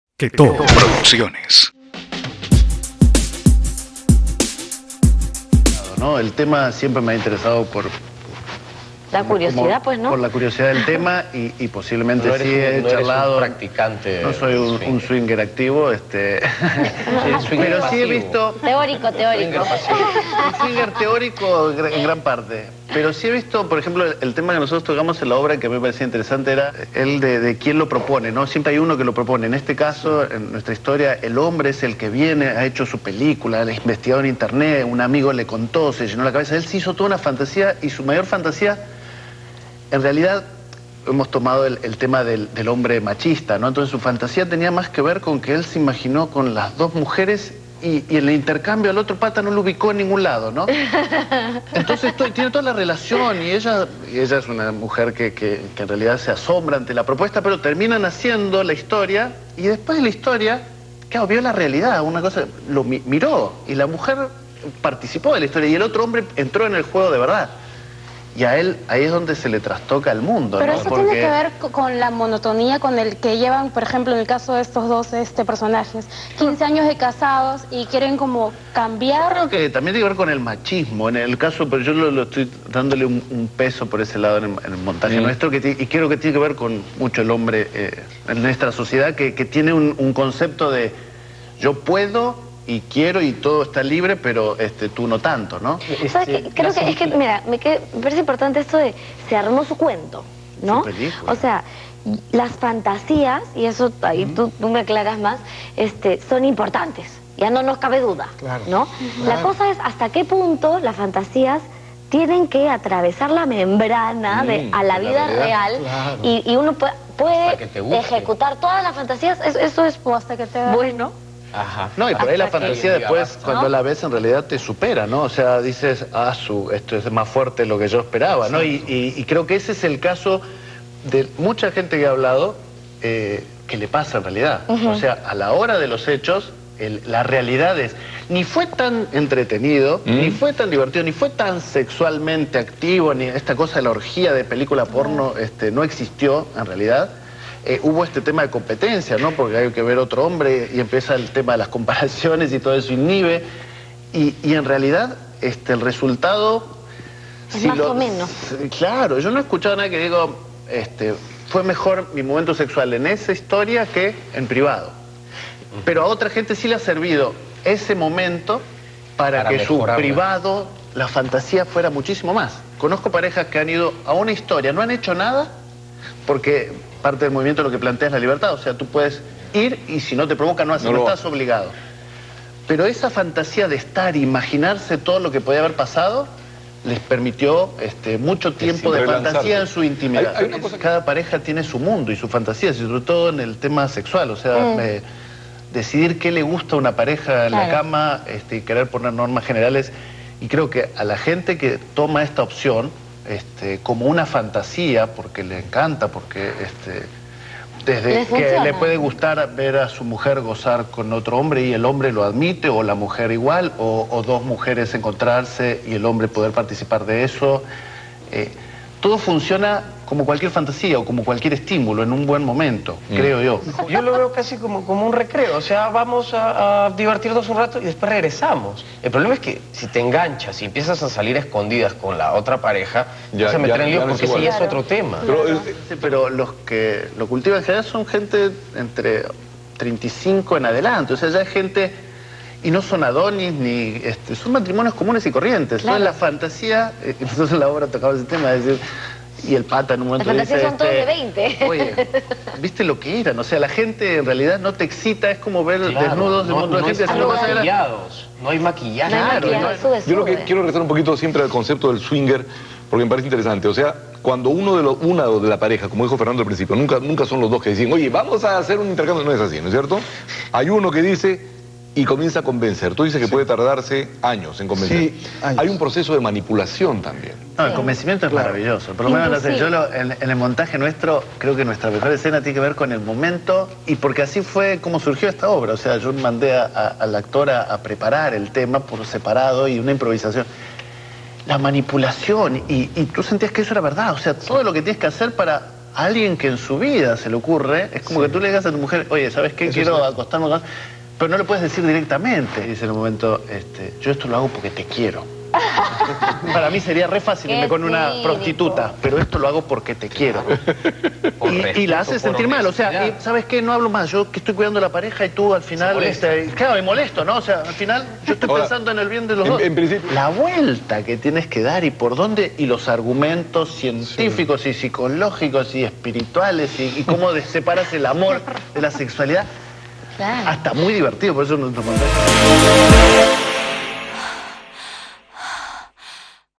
Swingers
Swingers, Montaje Teatral, entrevista en 3G
montaje producido por ketó producciones
Lima - Perú